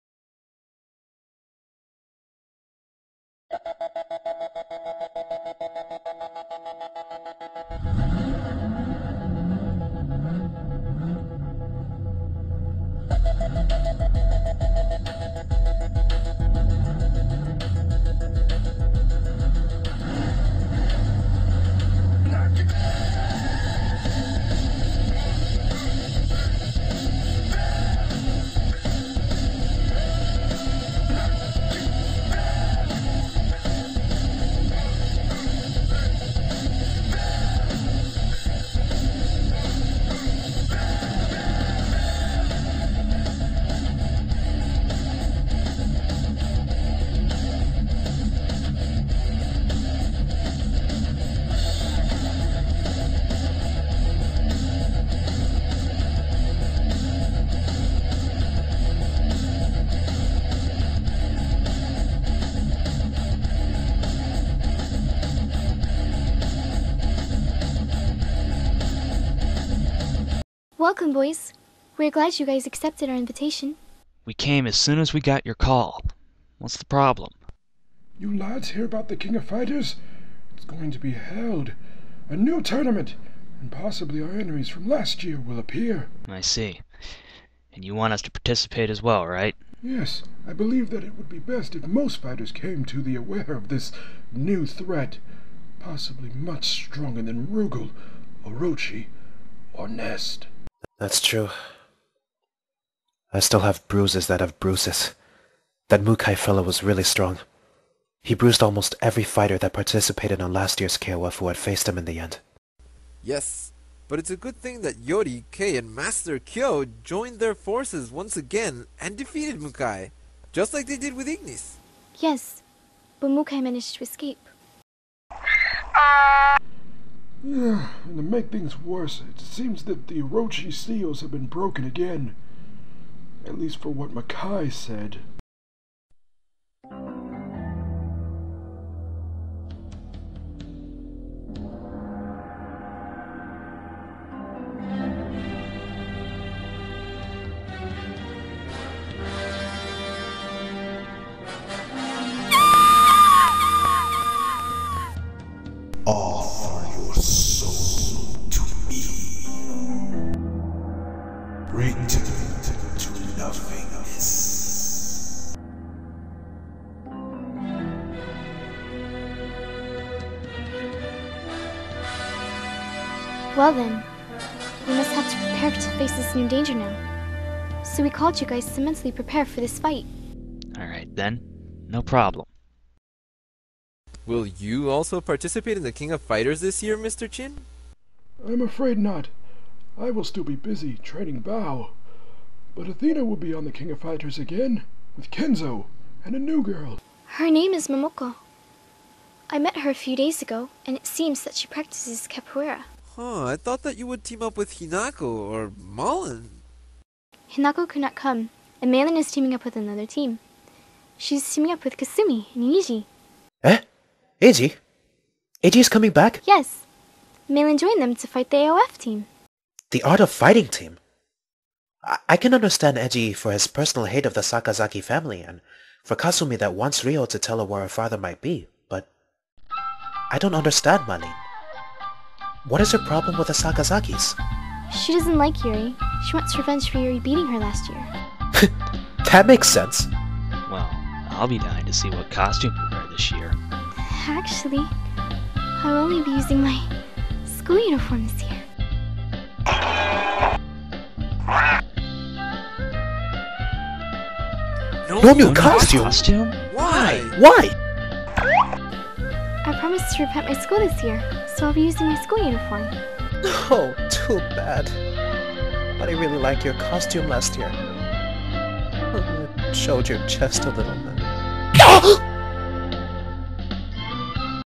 KOF The Team part 2
I decided to make a small KOF movie based on the events before KOF XI
especially saying how Kyo and Iori's team was formed on that game
but for that I decided to use some of the youtube users to play the voice of the characthers
and here they are

CAST:

ImmaRobotYo - Athena/Elisabeth
Anomaly188 - Kyo
IoriGaara - Iori/Chin
RurouniStarchild - Ash/Benimaru
Archedgar -- Shingo
SCRIK -- Orochi